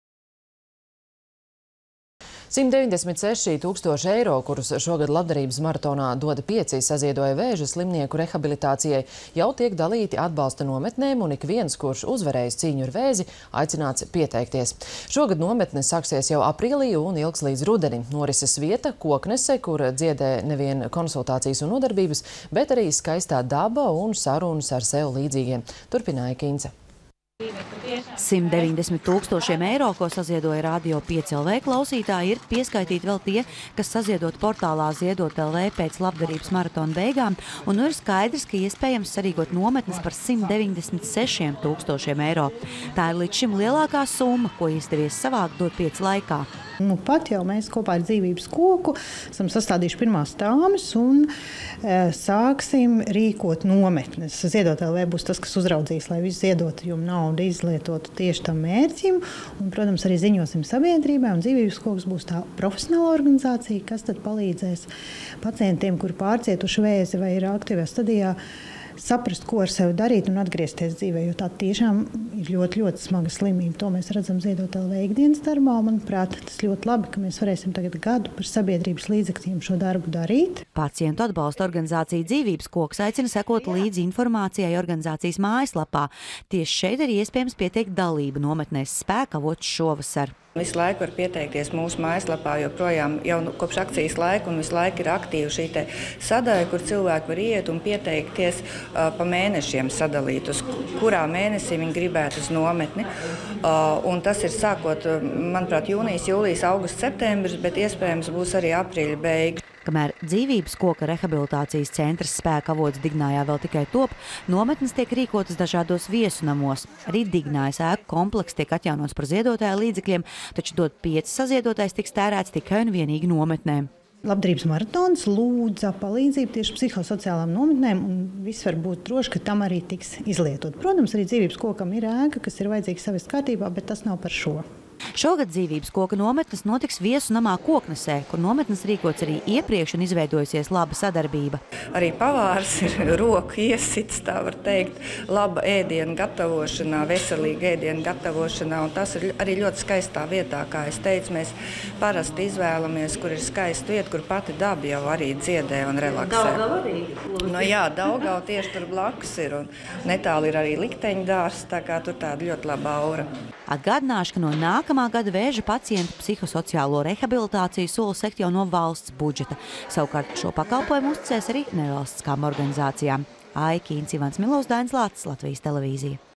Vēža pacientus aicina uz nometnēm
196 tūkstoši eiro, kurus šogad labdarības maratonā “Dod pieci” saziedoja vēža slimnieku rehabilitācijai, jau tiek dalīti atbalsta nometnēm, un ikviens, kurš uzvarējis cīņu ar vēzi, aicināts pieteikties! Šogad nometnes sāksies jau aprīlī un ilgs līdz rudenim. Norises vieta – Koknese, kur dziedē ne vien konsultācijas un nodarbības, bet arī skaistā daba un sarunas ar sev līdzīgiem. Turpina Aija Kinca.